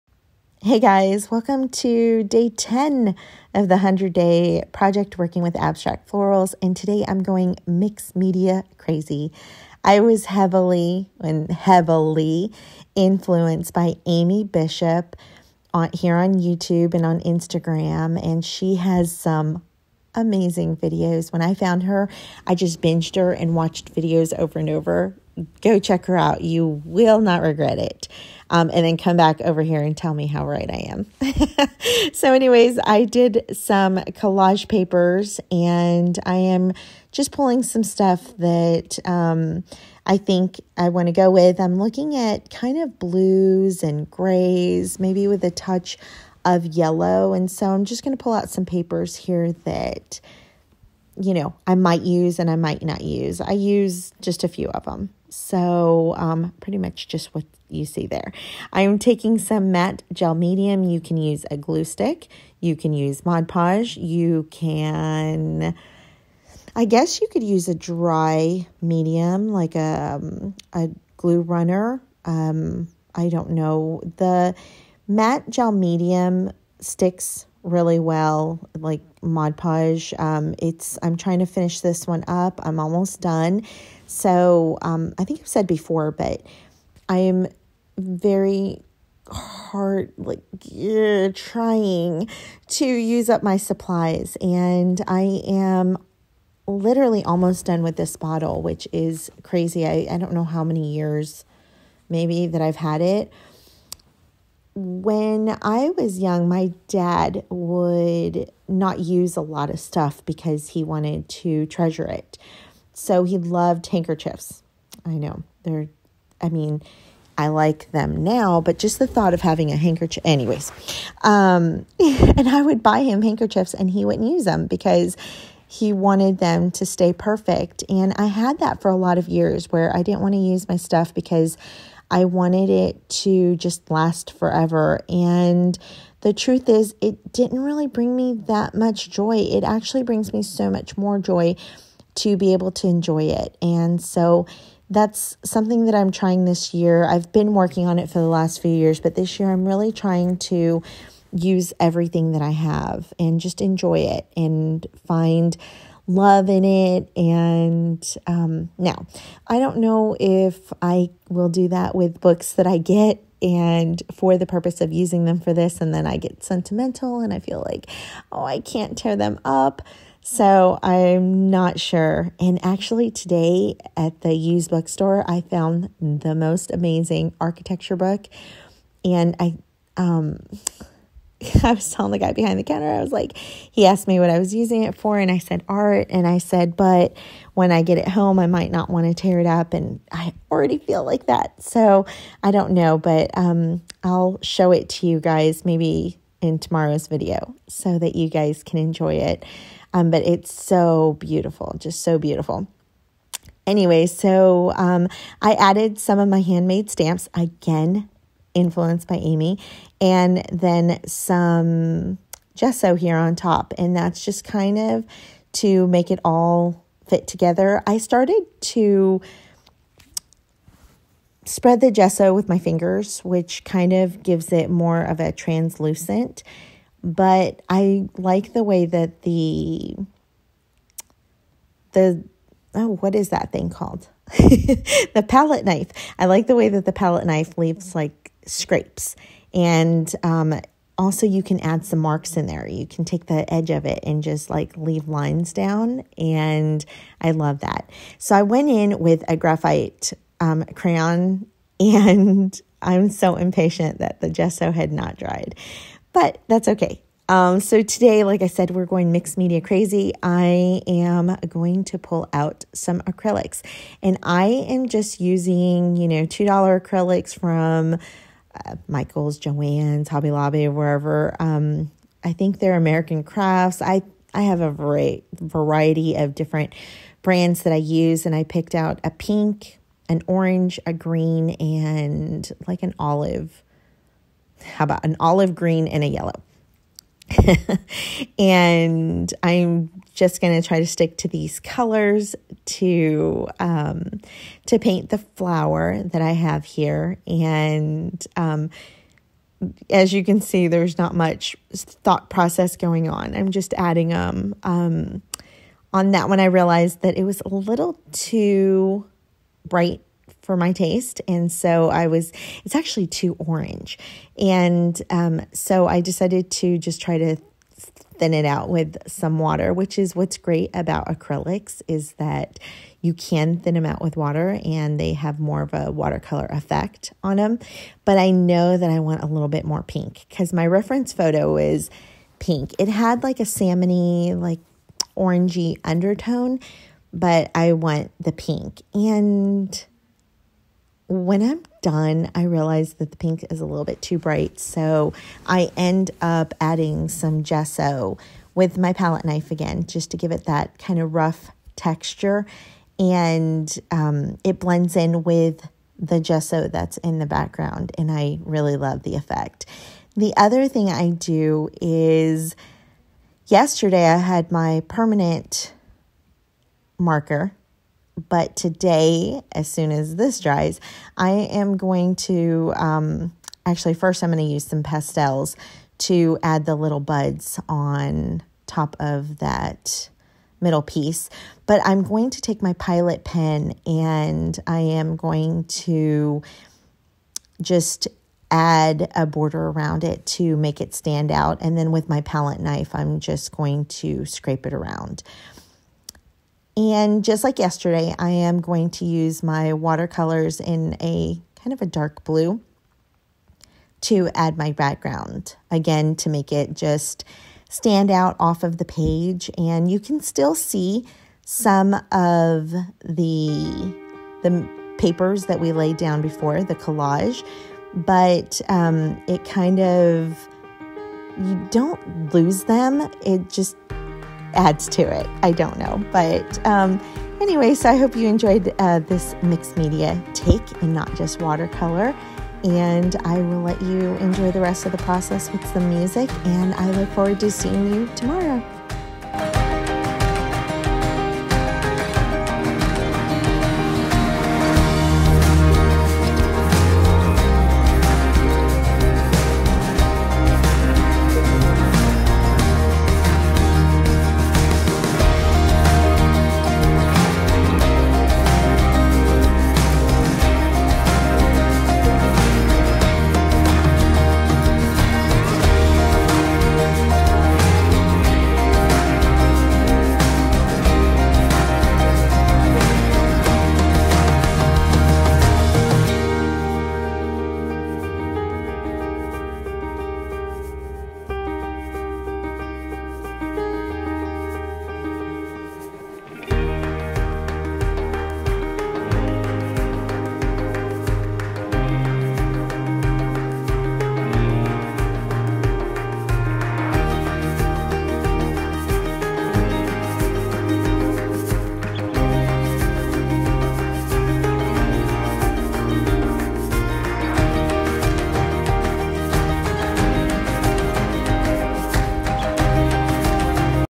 100 day project FLORALS Day 10: abstract florals TUTORIAL
Join me on the100dayproject as we delve into abstract florals. I’m taking this vintage microwave cookbook and working on florals for the next hundred days. This is a tutorial of how I make my abstract florals.

Products used:
gesso
matte gel medium
graphite pencil
acrylic paints
pastels
fountain pen
watercolors